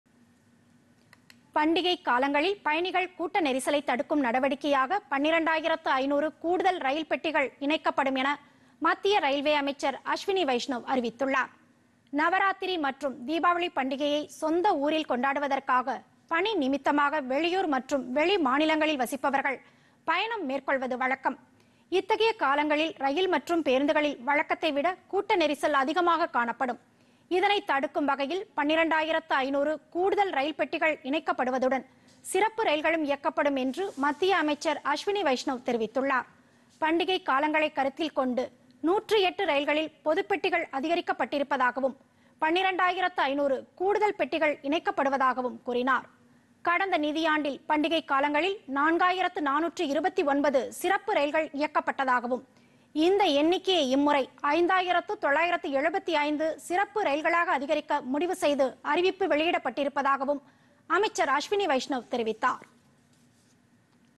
பண்டிகை நாட்களில் பயணிகள் கூட்ட நெரிசலை தடுக்க நடவடிக்கை- மத்திய அமைச்சர் அஸ்வினி வைஷ்ணவ்
பண்டிகை காலங்களில் பயணிகள் கூட்ட நெரிசலை தடுக்கும் நடவடிக்கையாக 12 ஆயிரத்து 500 கூடுதல் ரயில் பெட்டிகள் இணைக்கப்படும்  என மத்திய ரயில்வே அமைச்சர் அஸ்வினி வைஷ்ணவ் அறிவித்துள்ளார்.

       நவராத்திரி மற்றும் தீபாவளி பண்டிகையை சொந்த ஊரில் கொண்டாடுவதற்காக  பணி நிமித்தமாக வெளியூர் மற்றும் வெளி மாநிலங்களில் வசிப்பவர்கள், பயணம் மேற்கொள்வது வழக்கம்.

   இத்தகைய காலங்களில் ரயில் மற்றும் பேருந்துகளில் வழக்கத்தைவிட, கூட்டநெரிசல் அதிகமாக  காணப்படும்.

    இதனை தடுக்கும் வகையில், 12 ஆயிரத்து 500 கூடுதல் ரயில் பெட்டிகள் இணைக்கப்படுவதுடன்,  சிறப்பு ரயில்களும் இயக்கப்படும் என்று மத்திய அமைச்சர் அஸ்வினி வைஷ்ணவ் தெரிவித்துள்ளார்.

   பண்டிகை காலங்களை கருத்தில்கொண்டு, 108 ரயில்களில் பொது பெட்டிகள் அதிகரிக்கப்பட்டிருப்பதாகவும் 12 ஆயிரத்து 500 கூடுதல் பெட்டிகள் இணைக்கப்படுவதாகவும் கூறினார்.

    கடந்த நிதியாண்டில் பண்டிகை காலங்களில் 4 ஆயிரத்து 429 சிறப்பு ரயில்கள் இயக்கப்பட்டதாகவும், இந்த எண்ணிக்கையை இம்முறை 5 ஆயிரத்து 975 சிறப்பு ரயில்களாக அதிகரிக்க முடிவு செய்து அறிவிப்பு  வெளியிடப்பட்டிருப்பதாகவும் அமைச்சர் அஸ்வினி வைஷ்ணவ் தெரிவித்தார்.